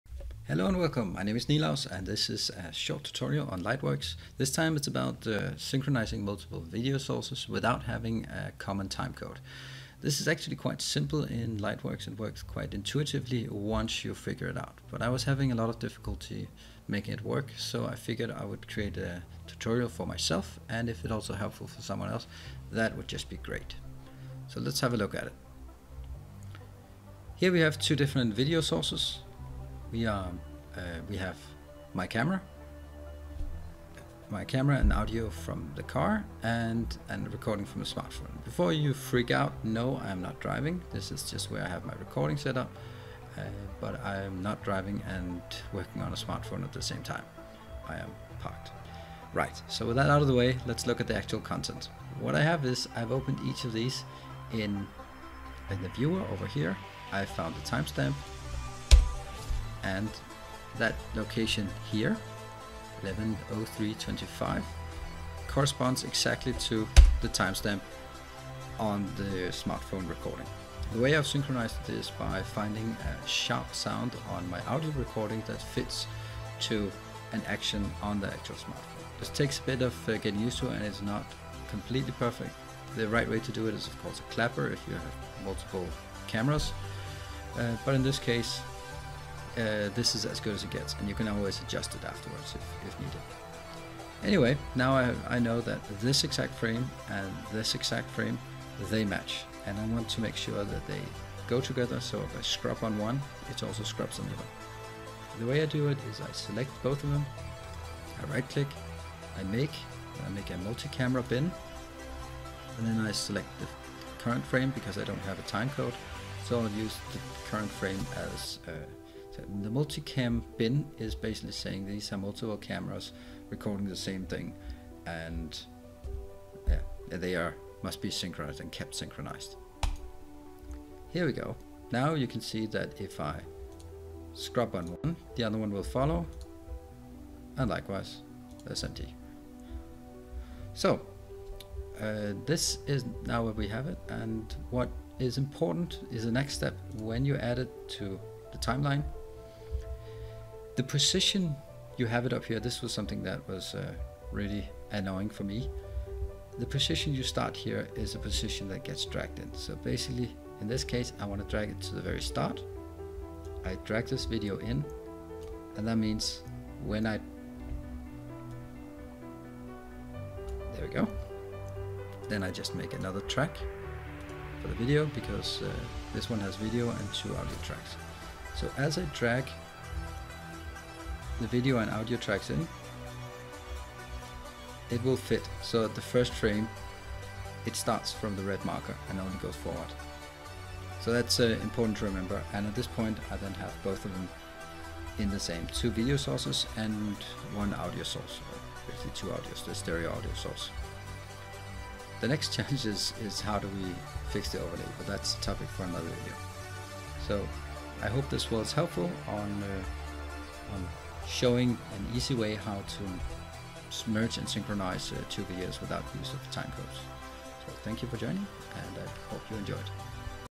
Lightworks: Synchronising Multiple Video Sources
Short tutorial on how to merge and syncronise several video sources by frames when you do not have shared time codes available.